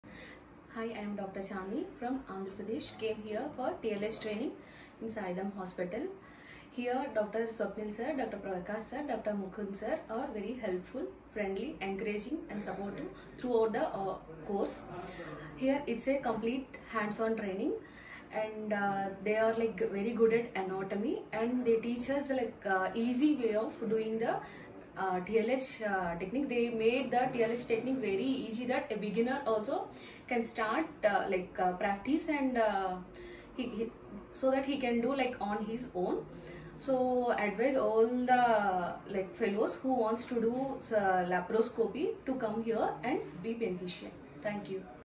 Dr M. Chandini -Srikakulam
Dr M Chandini (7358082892) from Srikakulam ( Andhra Pradesh) completed Gynaec endoscopy fellowship